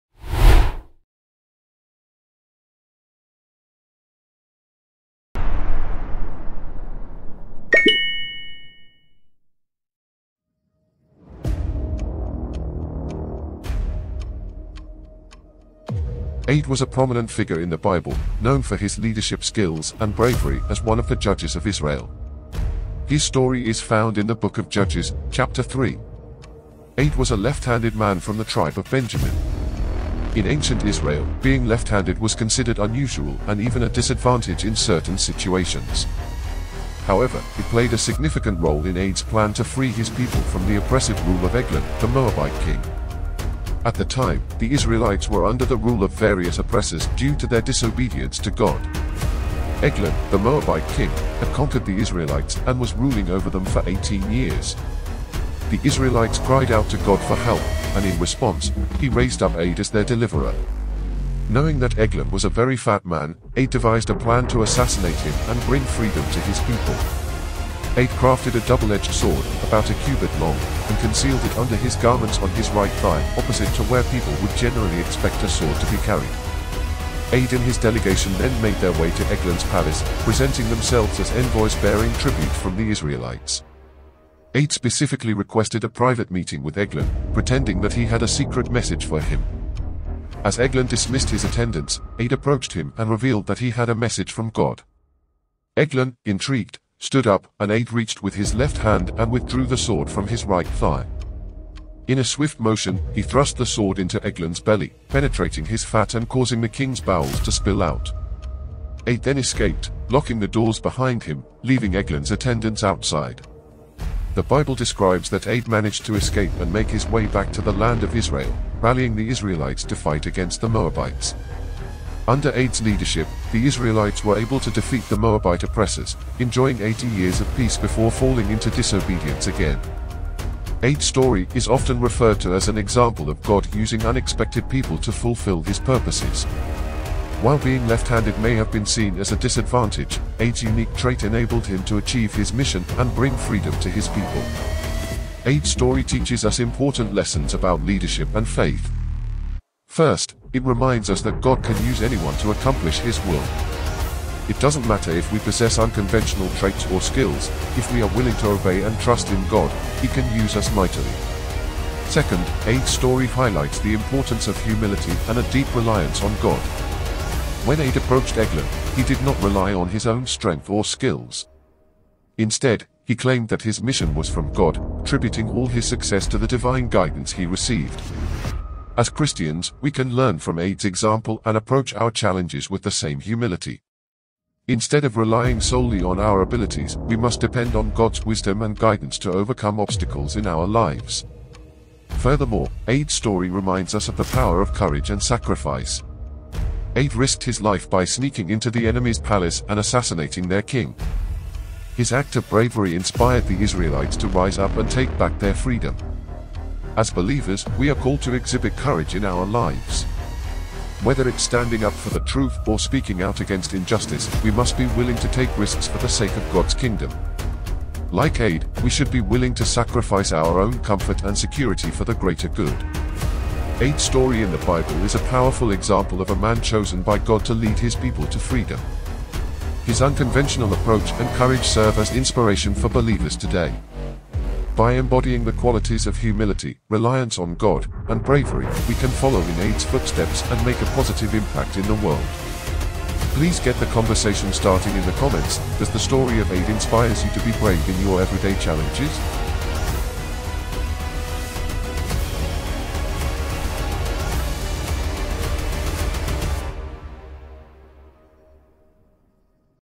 Who was the warrior Ehud ?
Ehud's story teaches us important lessons about leadership and faith. First, it reminds us that God can use anyone to accomplish His will. It doesn't matter if we possess unconventional traits or skills; if we are willing to obey and trust in God, He can use us mightily.

Second, Ehud's story highlights the importance of humility and a deep reliance on God. When Ehud approached Eglon, he did not rely on his own strength or skills. Instead, he claimed that his mission was from God, attributing all his success to the divine guidance he received.

As Christians, we can learn from Ehud's example and approach our challenges with the same humility. Instead of relying solely on our abilities, we must depend on God's wisdom and guidance to overcome obstacles in our lives.

Furthermore, Ehud's story reminds us of the power of courage and sacrifice. Ehud risked his life by sneaking into the enemy's palace and assassinating their king. His act of bravery inspired the Israelites to rise up and take back their freedom.

As believers, we are called to exhibit courage in our lives. Whether it's standing up for the truth or speaking out against injustice, we must be willing to take risks for the sake of God's kingdom. Like Ehud, we should be willing to sacrifice our own comfort and security for the greater good.

Ehud's story in the Bible is a powerful example of a man chosen by God to lead his people to freedom. His unconventional approach and courage serve as inspiration for believers today. By embodying the qualities of humility, reliance on God, and bravery, we can follow in Ehud's footsteps and make a positive impact in the world.

Please get the conversation starting in the comments, does the story of Ehud inspires you to be brave in your everyday challenges ?